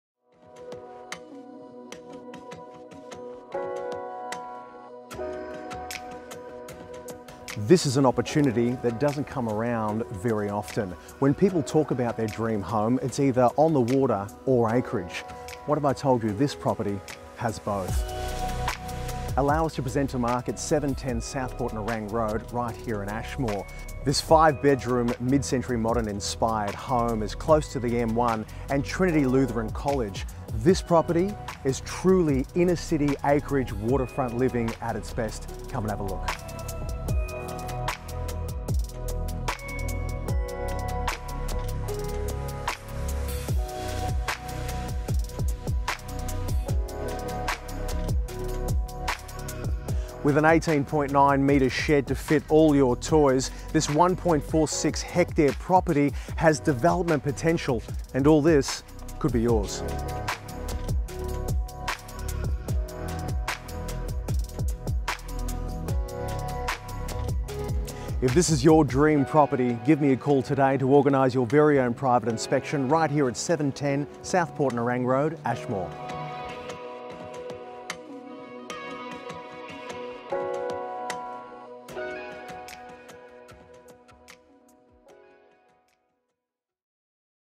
710 Southport - Nerang Road, Ashmore.
ACREAGE LIVING AT ITS ABSOLUTE FINEST

When deciding on a ‘dream home’, most people dream of either an acreage for peace and privacy or waterfront for water access and lifestyle…. What if we told you, you could have both!!!

Opportunities like this are rare….. 1.46 hectares of land….. stunning 5 bedroom residence….. 40 meters of water frontage on the Nerang River…. Central location…… and development potential…. honestly, what more could you want?

This magnificent property will suit the lifestyle buyer wanting to take advantage of inner city acreage living with over 40 meters frontage on the Nerang River all to themselves; Or the astute investor with the development potential to subdivide this vast property into as many as 8 lots.

Whatever way you decide to go, this opportunity is here for the taking…

Exisiting Home:
-Architecturally designed Mid-Century Modern style home complete with split level and potential to renovate into trendy Palm Springs Style Stunner
-5 spacious bedrooms, including generous master with walk in robe and ensuite.
-3 separate indoor living areas, making it ideal for the growing family.
-Large covered deck spanning the entire side of the home, perfect spot to enjoy the gorgeous views of rolling greens and the Nerang River.
-Large double lock up garage with secure internal access and spacious storage room
-18 x 9m shed with high clearance ceilings, 4 bays with roller doors, recreation room and patio.
-Solar system

Lifestyle:
-1.46 hectares of manicured land fronting the beautiful Nerang River.
-Northern end of site measures over 60 meters wide providing plenty of privacy from neighbours
-Ideal lifestyle for boaties, waterskiers, jet skiers or anyone who loves to live by the water.
-Land large enough to accommodate up to 2 horses.
-Large shed/man cave/she shed/ home business or just perfect to safely house all the ‘toys’ (motocycle, boats, campers, jet skis, trailers ect).

Development potential
-8 lot subdivision
-Existing home would be retained on 1213m2 lot
-5 additional lots ranging from 601m2 to 667m2
-2 large waterfront blocks with over 20 meters of water frontage for each, lot 7 – 3878m2 and lot 8 – 3505m2
** A pre-lodgement meeting with GCCC has shown support for 8 lots however development is yet to be approved.